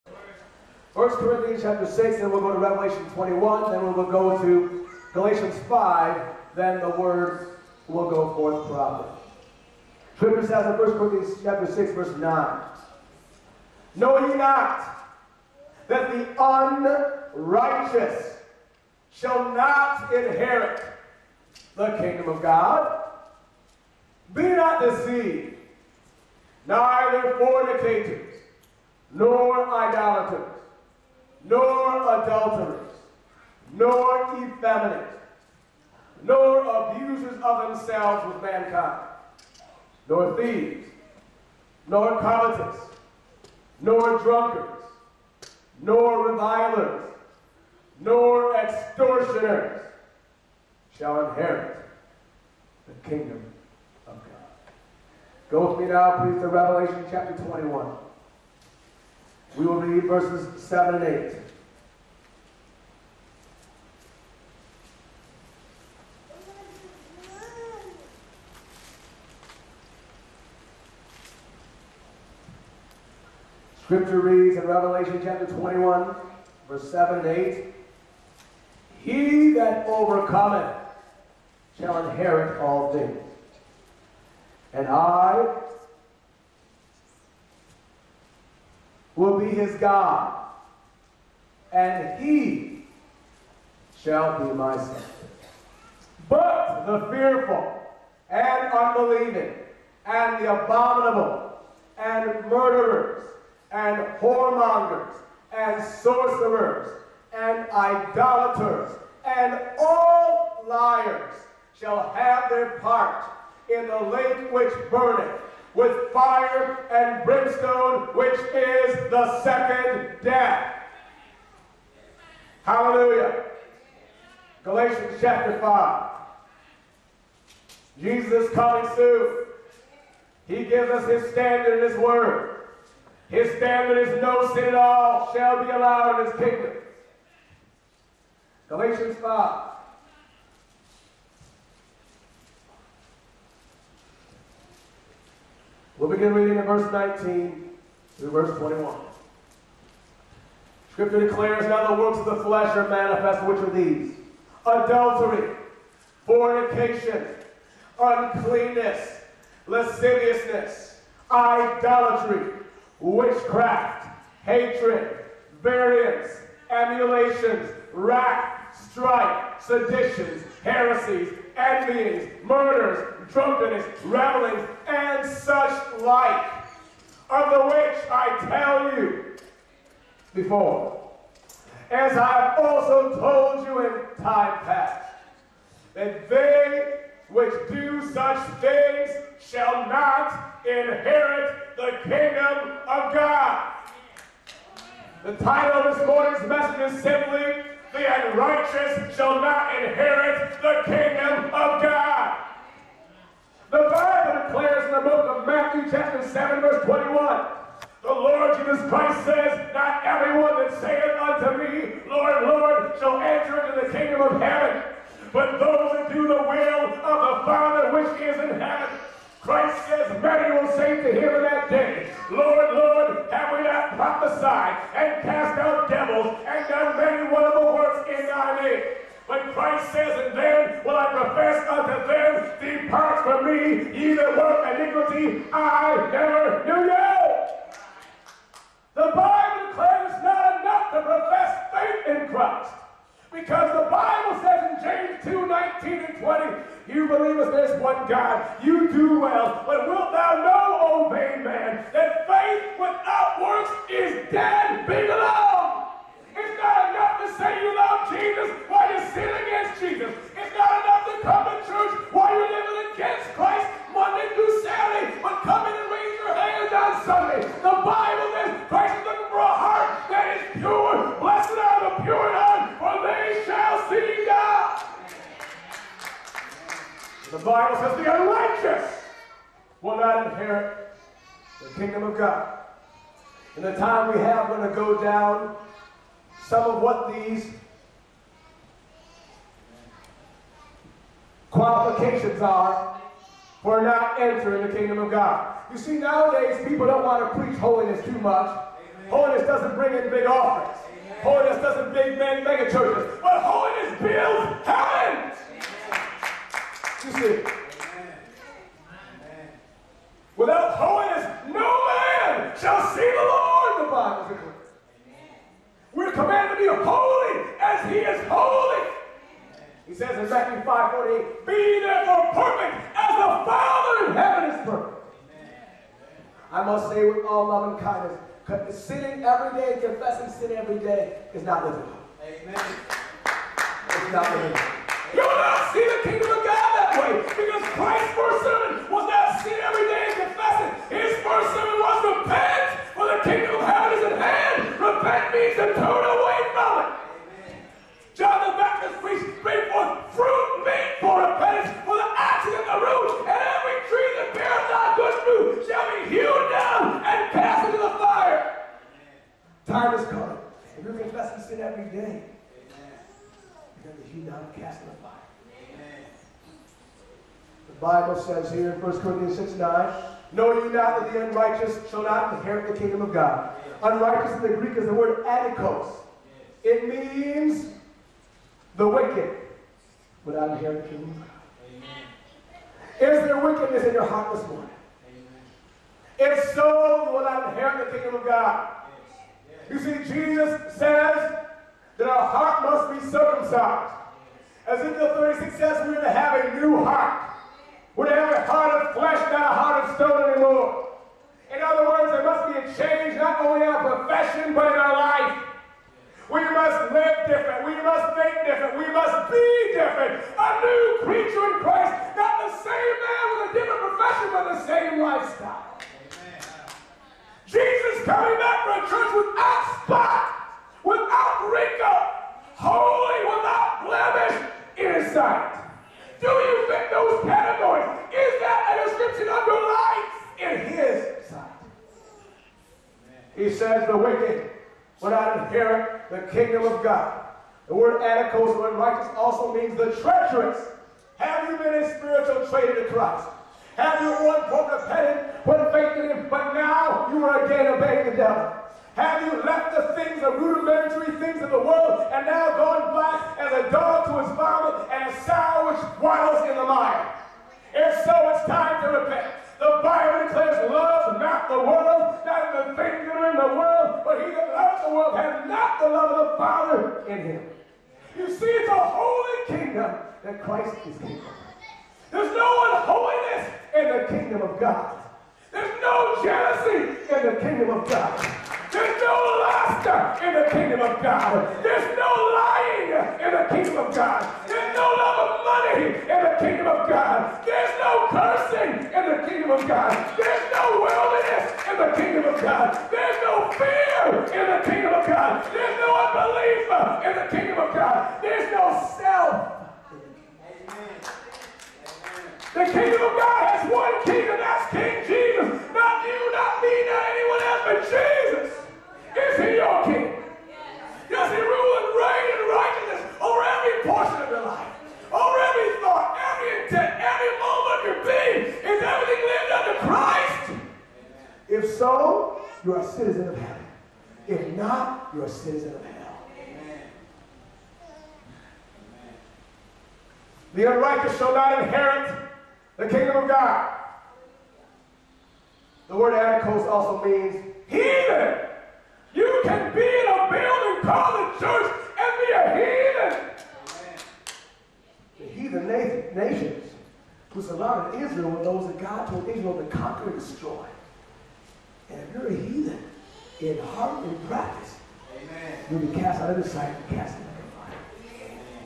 "The Unrighteous Shall Not Inherit the Kingdom of God" Pt1
Michael Venyah preaching in Beaumont, Texas.
"The Unrighteous Shall Not Inherit the Kingdom of God" (Part 1 of 5)